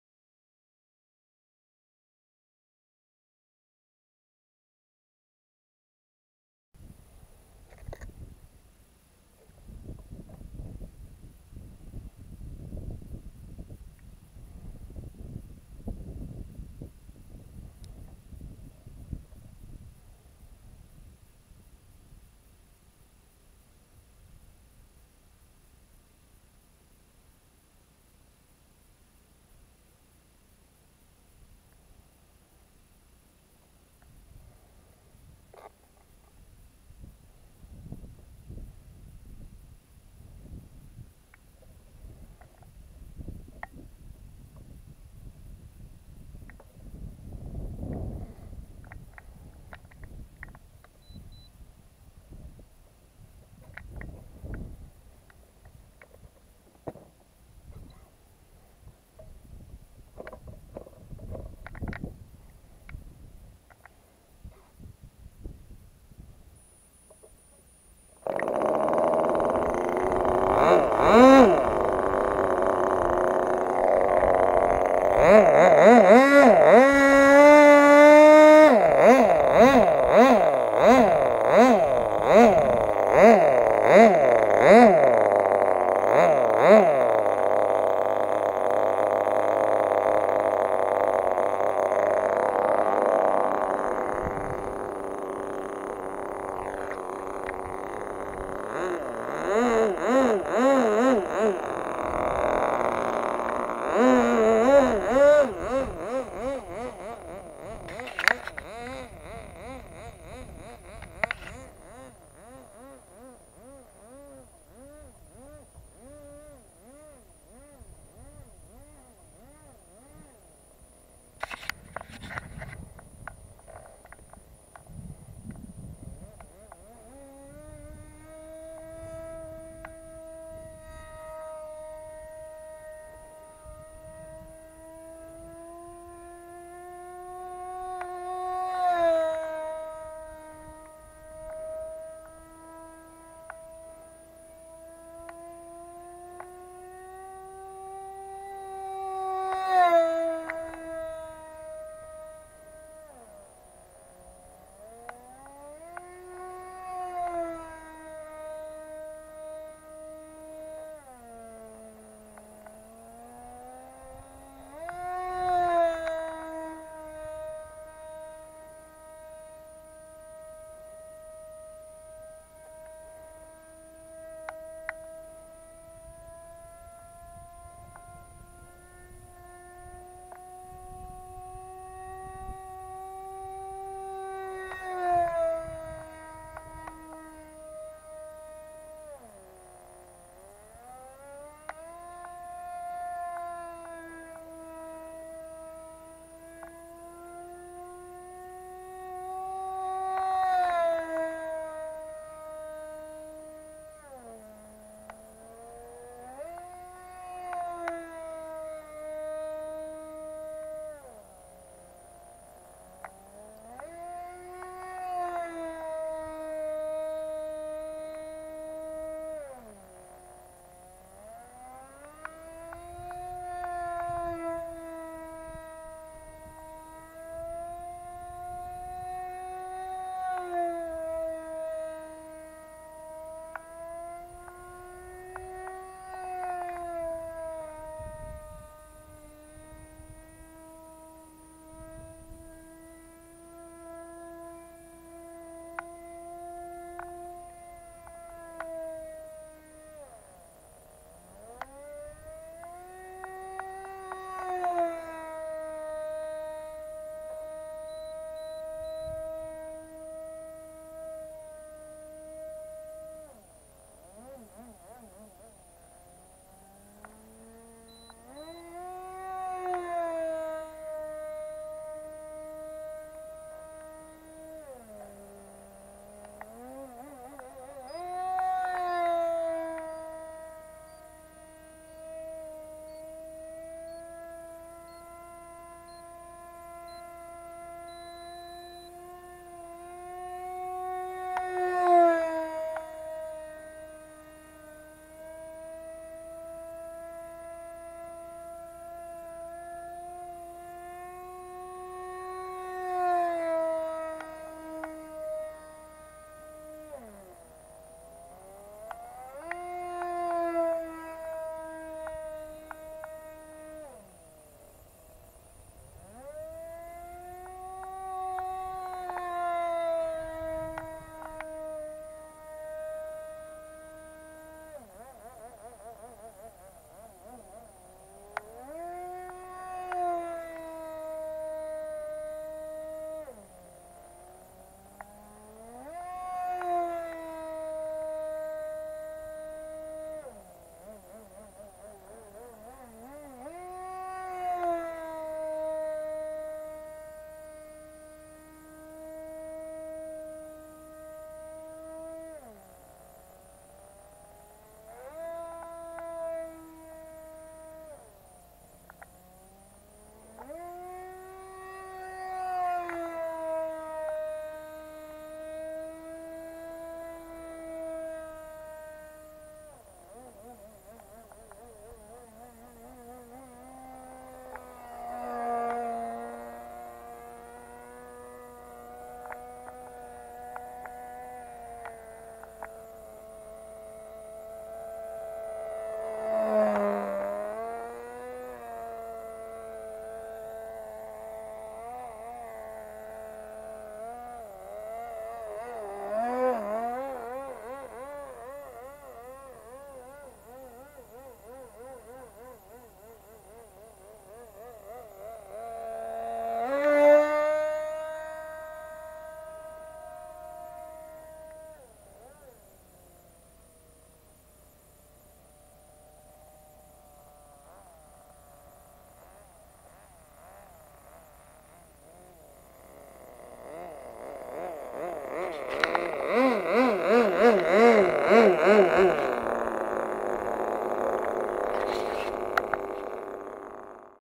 Engine MP-JET .061 r/c (The weather is better than on the previous day's flight)
Engine of Czechoslovakian origin and great performance.
Propeller: APC 6 x 3
Model: Mini Bird
Channel: 4